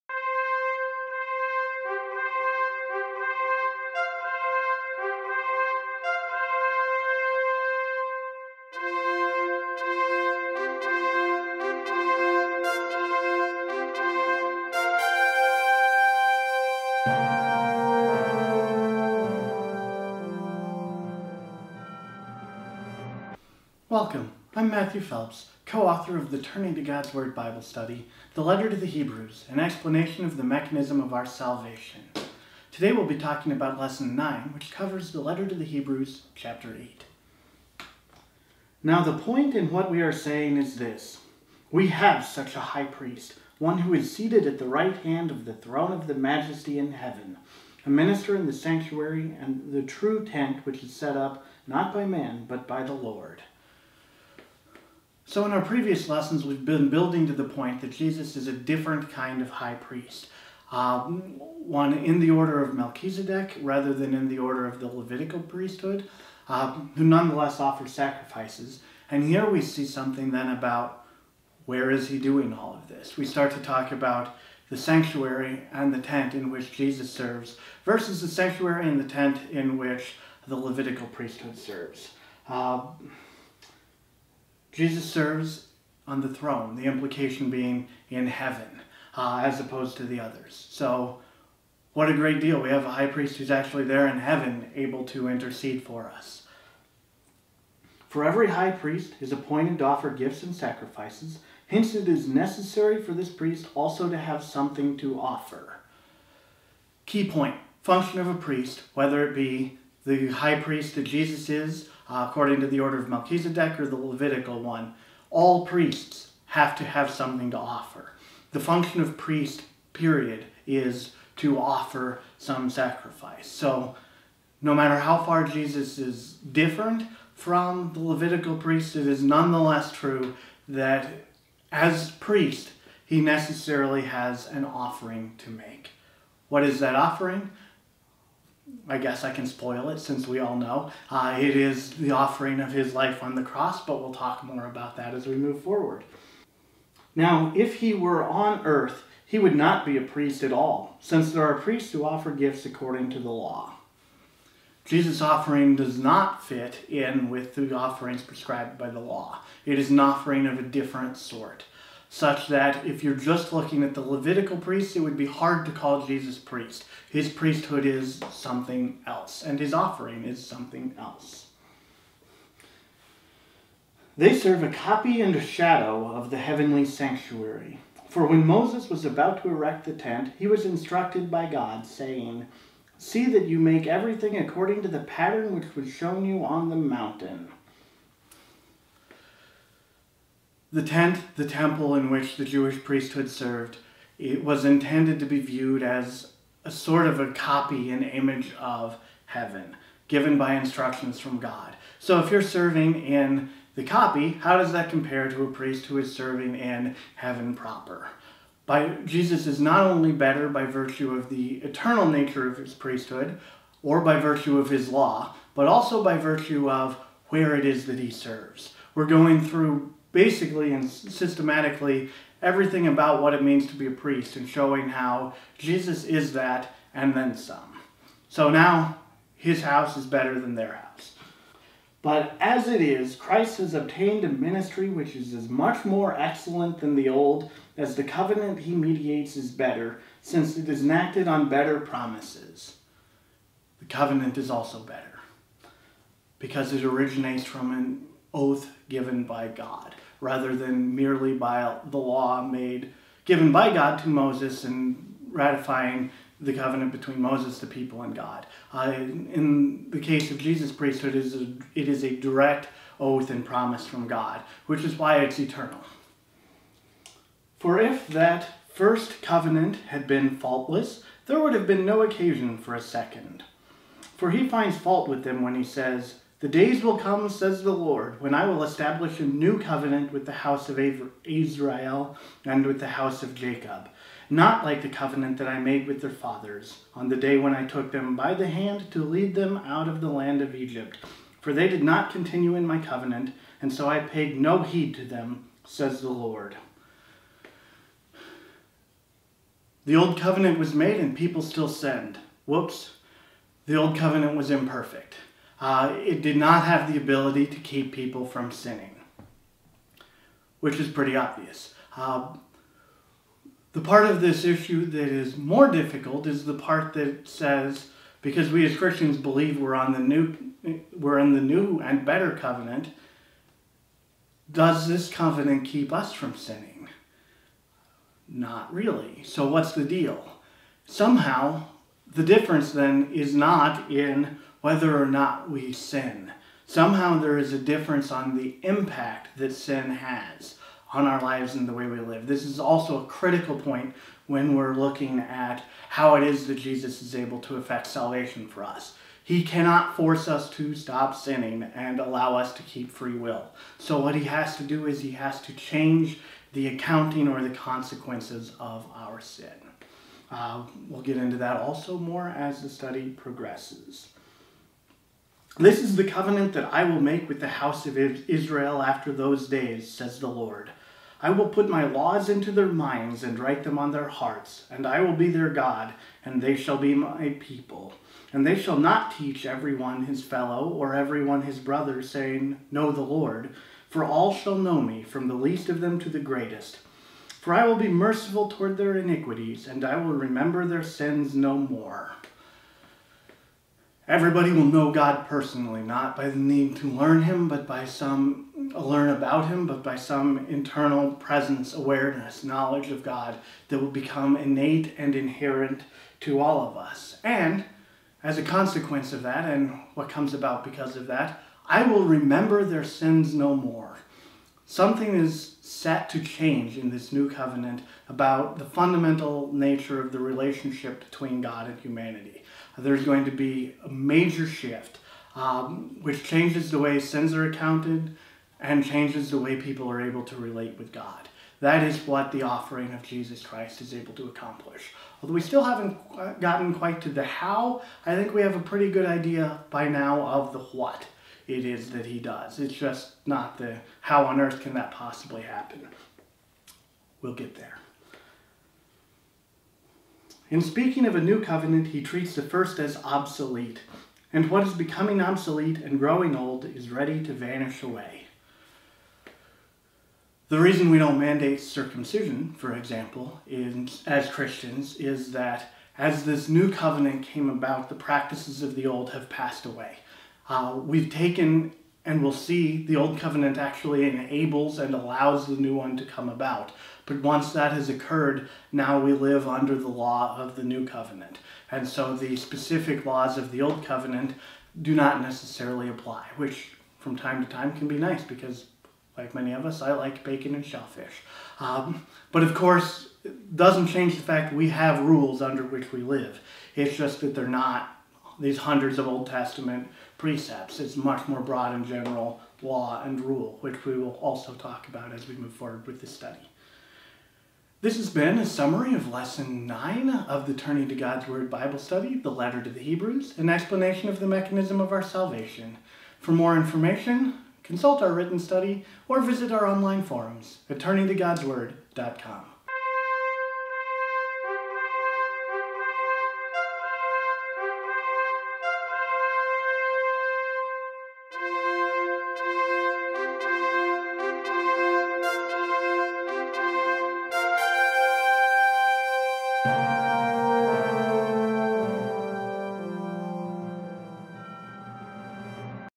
Letter to the Hebrews: Lesson 9
Follow along with author Matthew Phelps as he discusses Hebrews 8 in "Enacted on Better Promises," Lesson 9 in The Letter to the Hebrews: An Explanation of the Mechanism of Our Salvation.

Supplemental commentary, resources, and reflection questions can be found on our